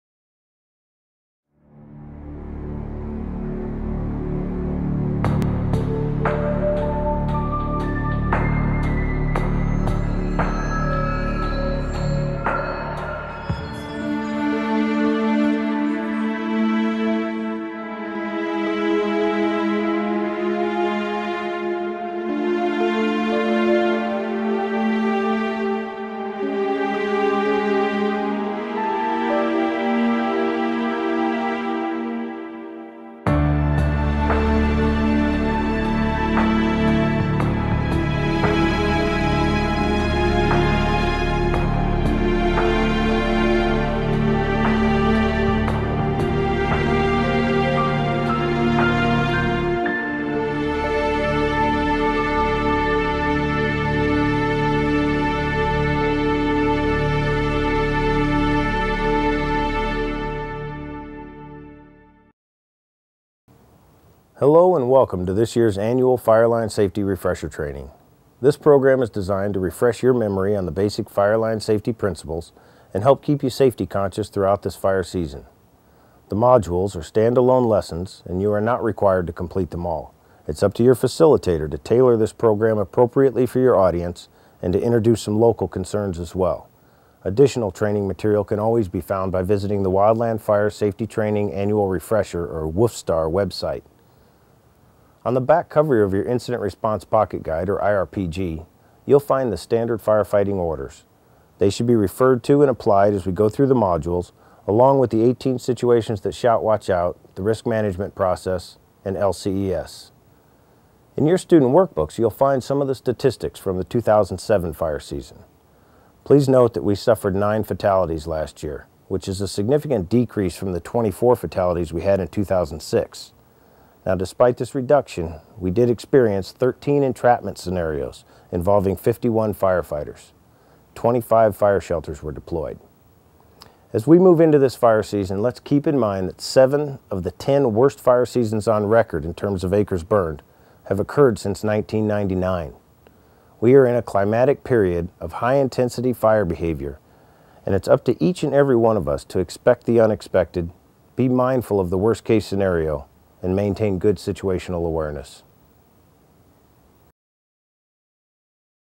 Changing Fire Environment - Part 1 - Introduction
Changing Fire Environment - Part 1 - Introduction - National Interagency Fire Center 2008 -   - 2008 Fireline Safety Refresher Training.